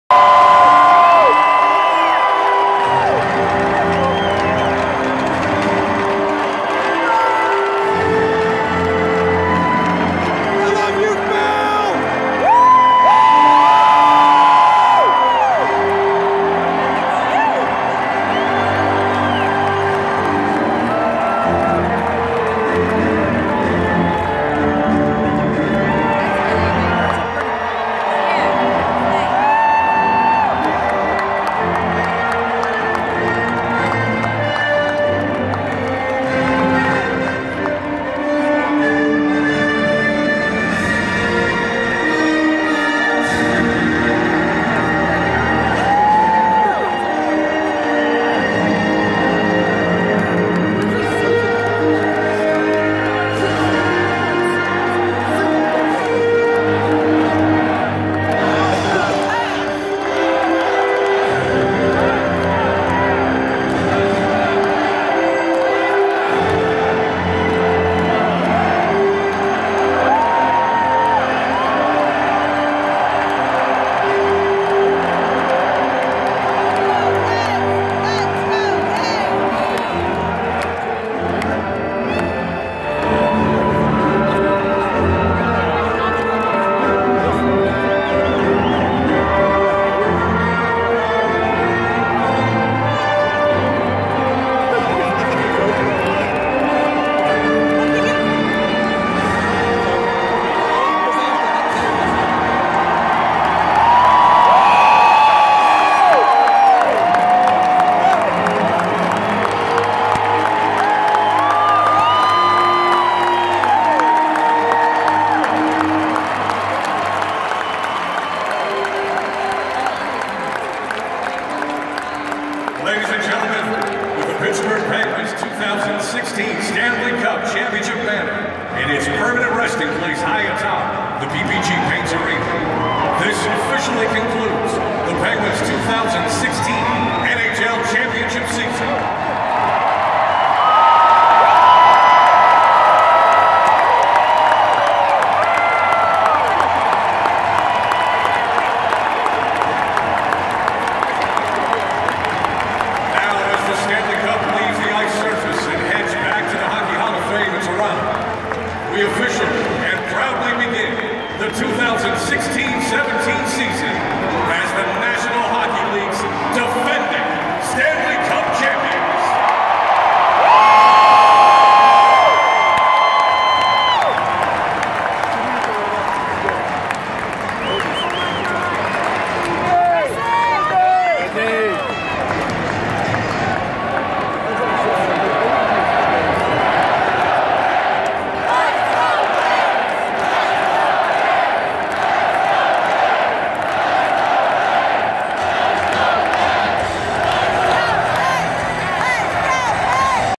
Pittsburgh penguins 2016 Stanley Cup celebration opening night
Pittsburgh penguins Stanley Cup celebration banner opening night 2016